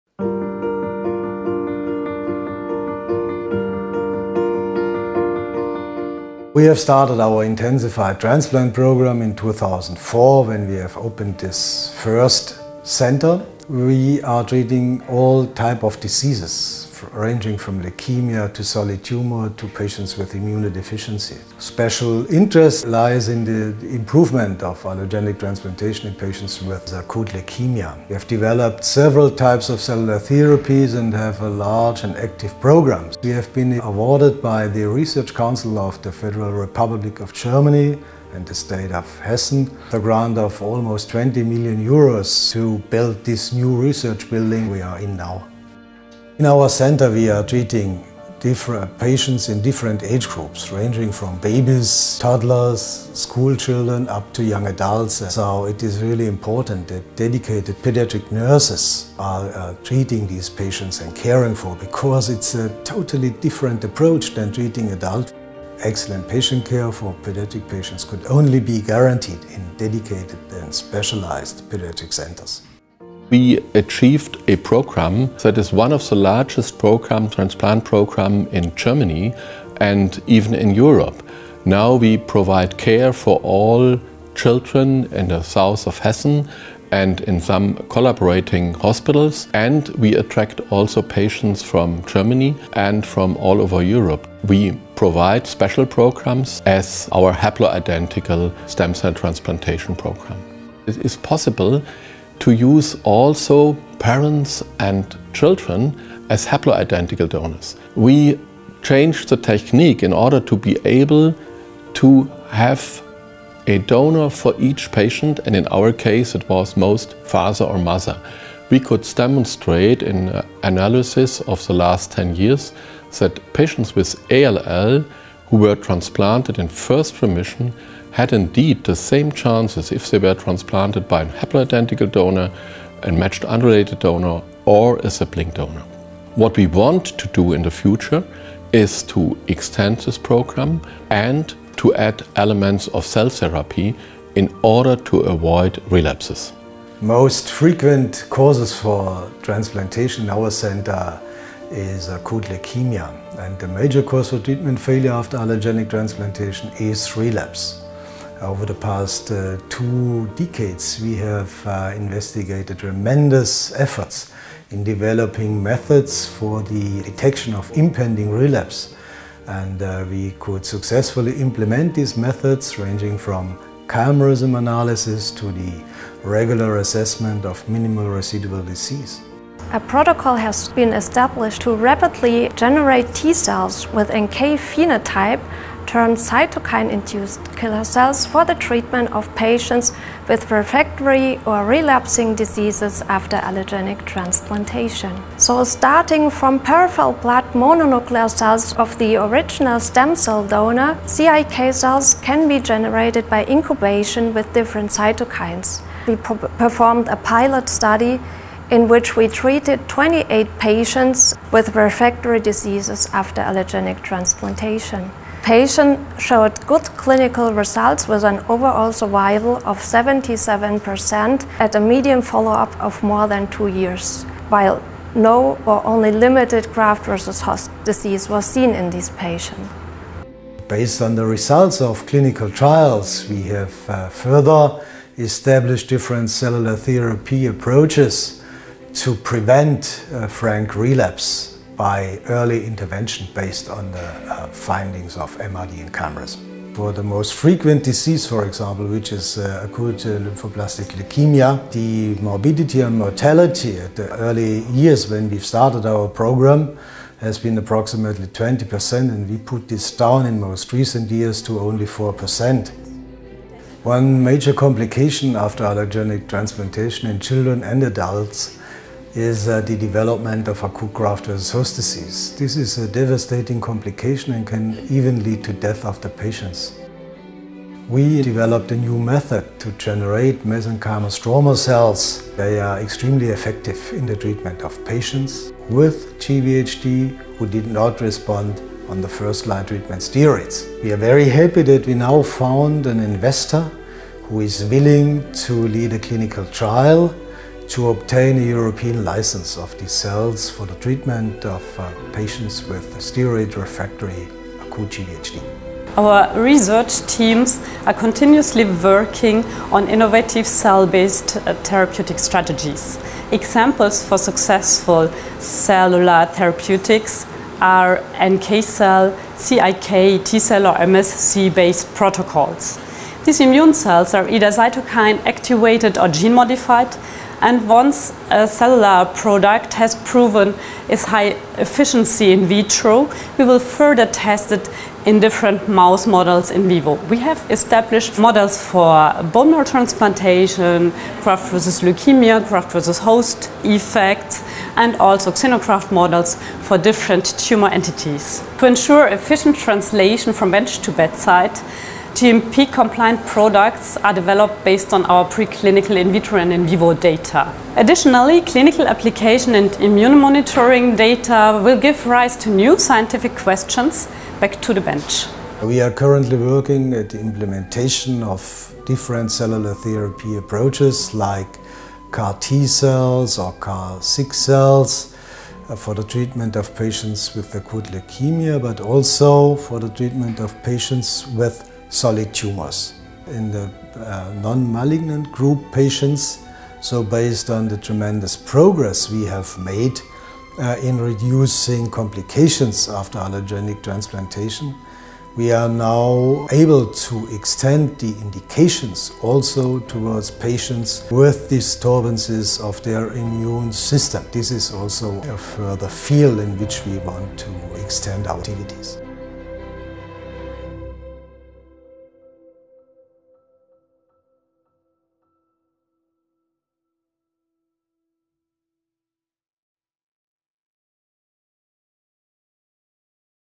Excellent patient care for children and adolescents
University Hospital Frankfurt, Goethe University, Department for Children and Adolescents, Division for Stem Cell Transplantation and Immunology focuses on  excellent patient care for children and adolescents in an appropriate environment by dedicated nurses.
The Division for Stem Cell Transplantation stresses importance of having a suitable donor for all their patients and runs a successful haploidentical stem cell program. Relapse is the major cause for treatment failure after allogeneic stem cell transplantation in patients with acute leukemia the division has a program to identify and treat patients with impending relapse using MRD and chimerism analysis. Based on these findings the division runs a cellular immunotherapy program using different cell types including CART cells to treat and prevent relapse as well as Mesenchymal Stromal Cells program to treat aGVHD.